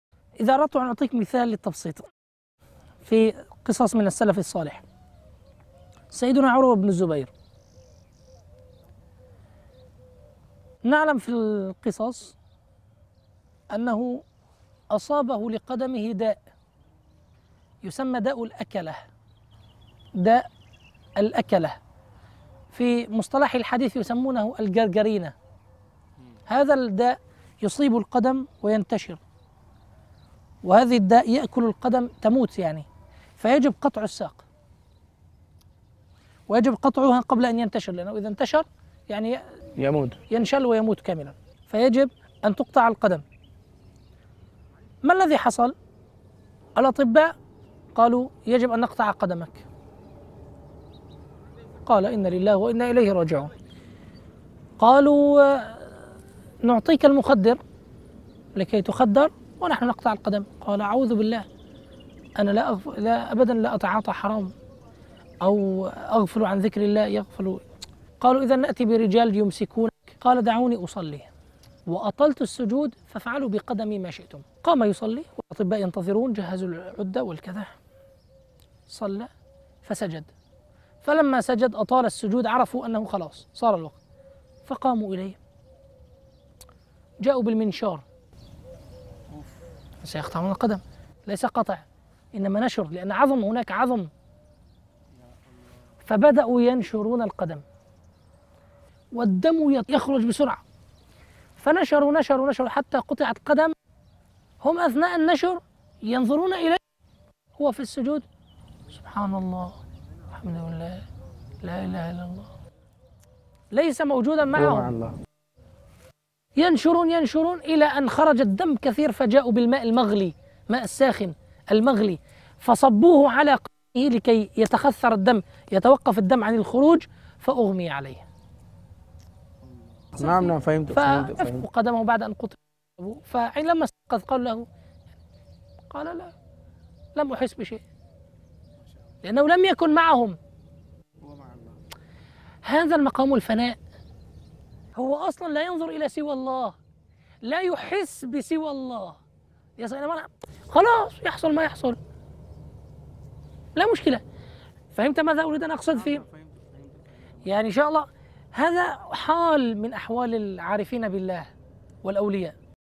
مثال مقام الفناء في الله
مقطع من الحلقة التاسعة من سلسلة نفحات رمضان: فیه بيان مثال مقام الفناء في الله

لمشاهدة المزيد من الفيديوهات االاسلامية شاركوا في قناتنا على اليوتوب